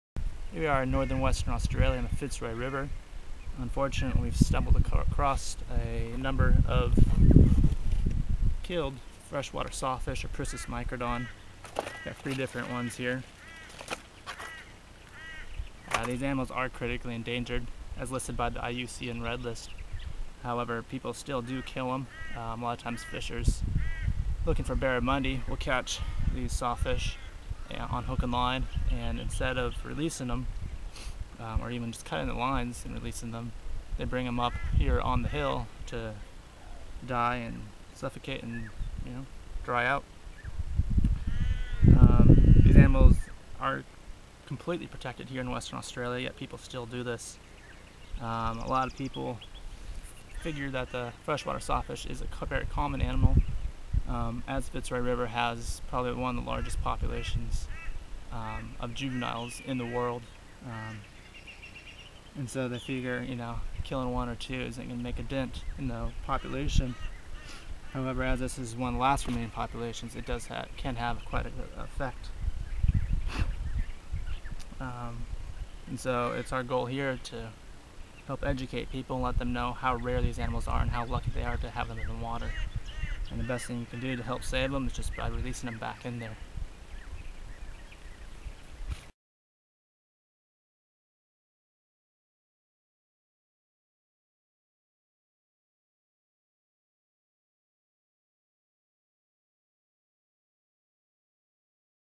Reckless killing of sawfish.wmv
Scientist Jeff Whitty discovers and discusses the reckless killing of three freshwater sawfish (Pristis microdon) found in northern Australia